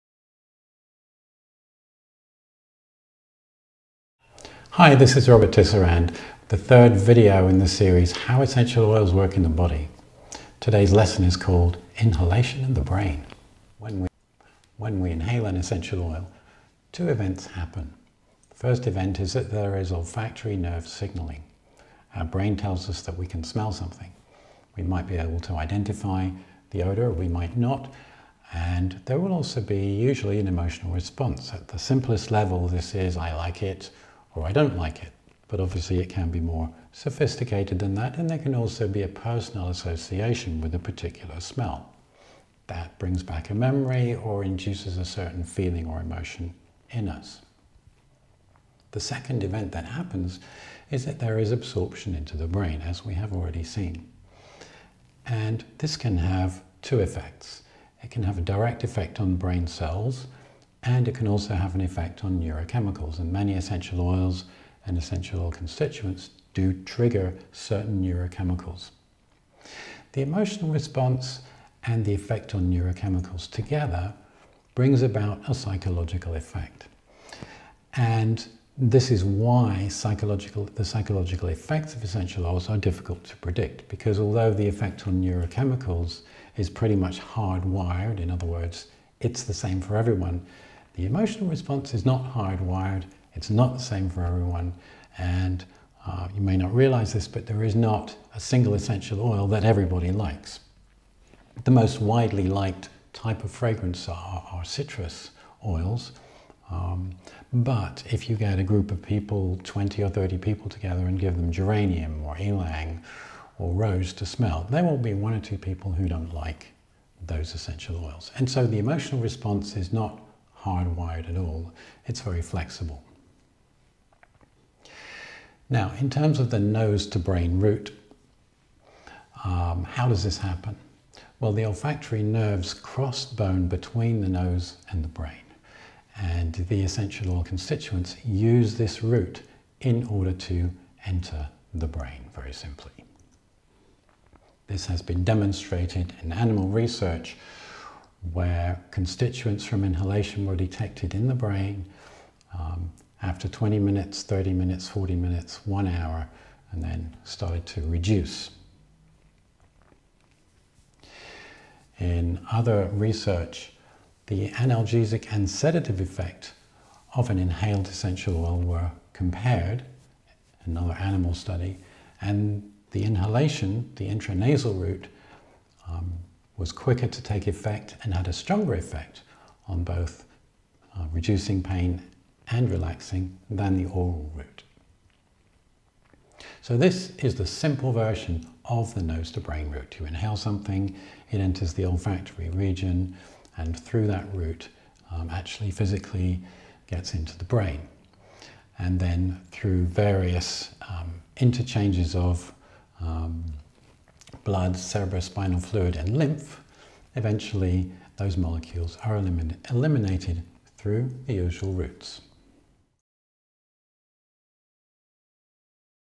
How Essential Oils Work in the Body 3: Inhalation and the brain
Welcome to the third lesson in the mini-course!

Today, Robert Tisserand will tell you how inhaling essential oils influences our brain.